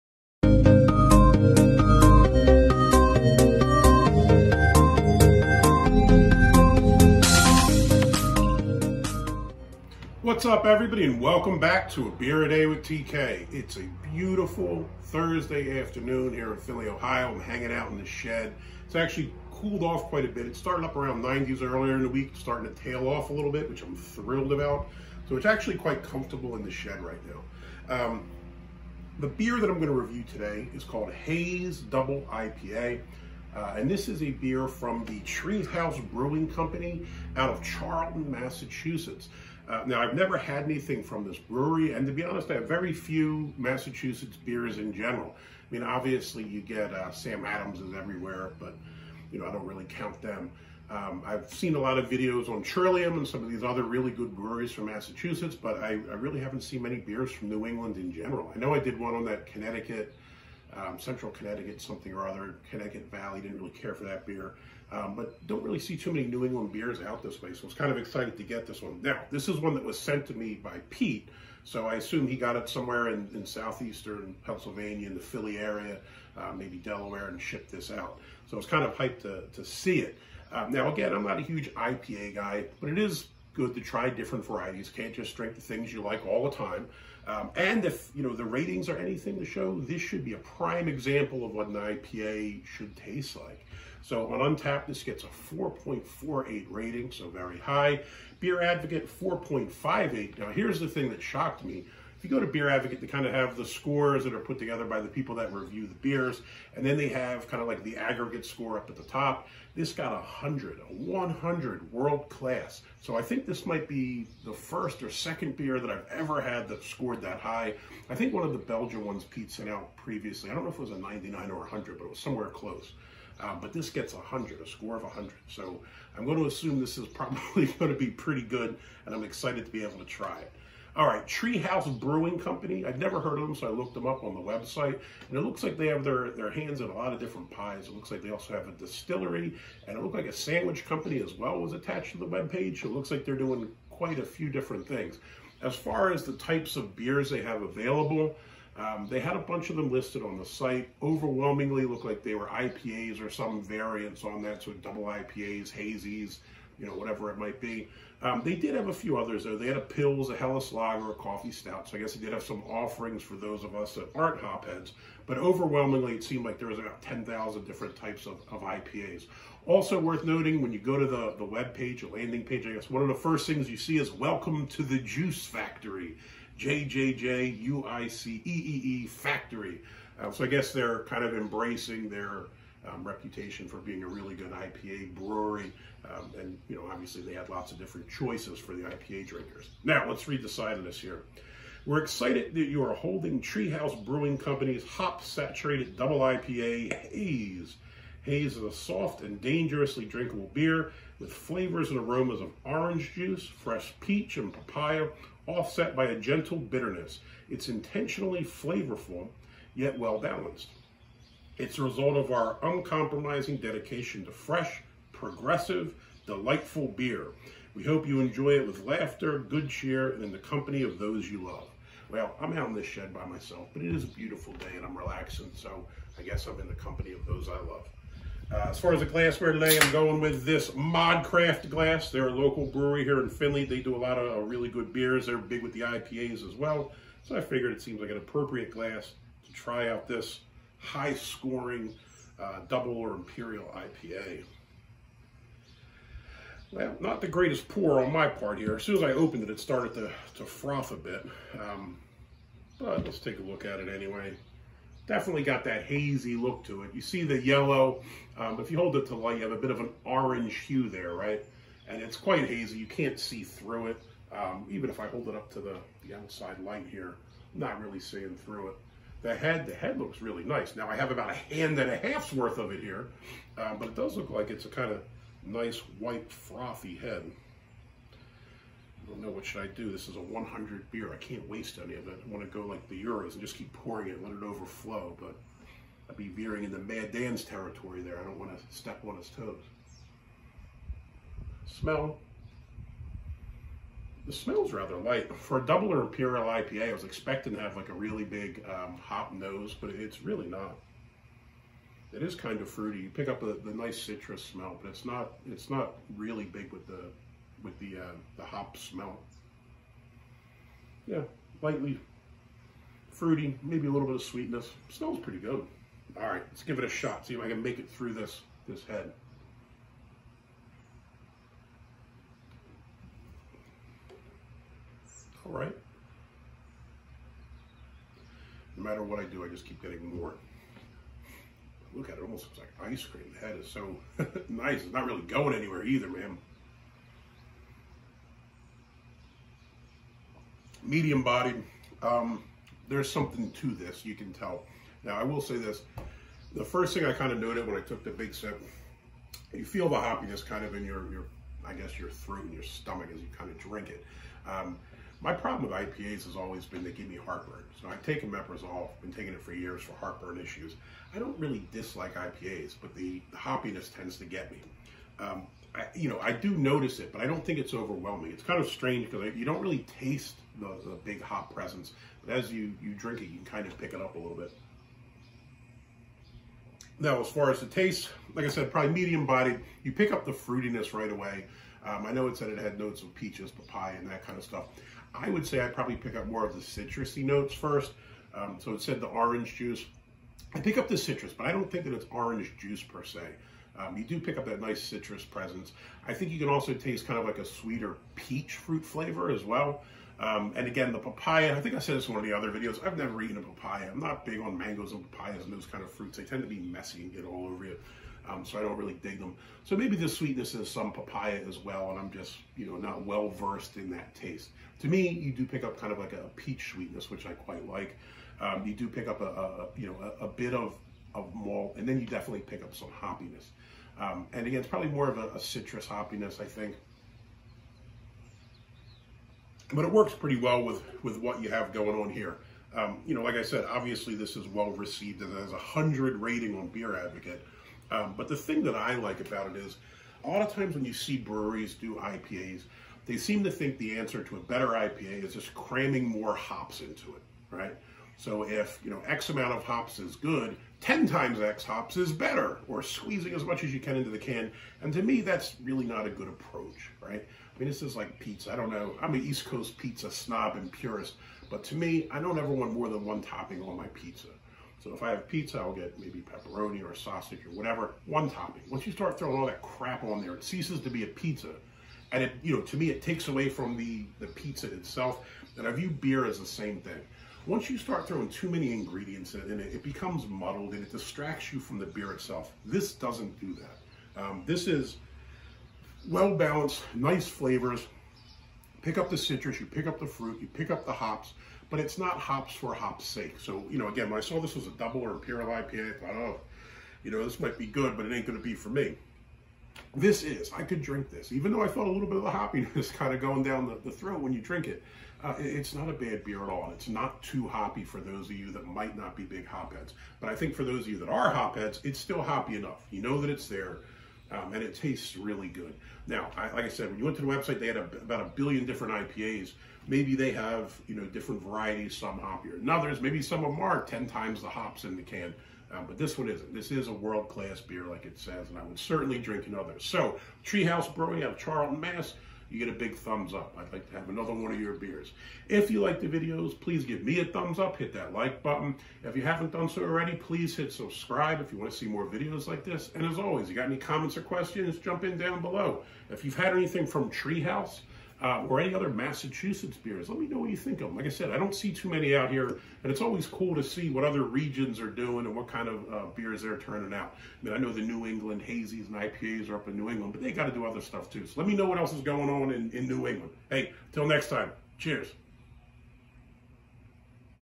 Day 318 Beer Review: Haze, Tree House Brewing, Double IPA, 100% World Class Score on BeerAdvocate!!
Day 318 of 365: Haze, Tree House Brewing Co., Charlton, Massachusetts.
Purchase: Gift from Pete.

It is not everyday that you get to review a beer with a perfect 100 "World Class" rating on BeerAdvocate, but thanks to Pete, I have the privilege of giving this Double IPA a try. This one comes in at 8.2% ABV with 90 IBUs and poured a hazy yellowish color with an orange hue. The head was abundant, frothy and looked wonderful, despite my terrible pour. The nose was much more subtle than I had expected from a double IPA, and you could pick up citrus notes as well as a bit of hops. This was medium bodied, and you could pick up citrus fruit, as well as some peach sweetness. The hops were present, but as a complimentary flavor to the fruit. Overall, this was a really good beer and I am thankful Pete sent it out so I could give it a shot!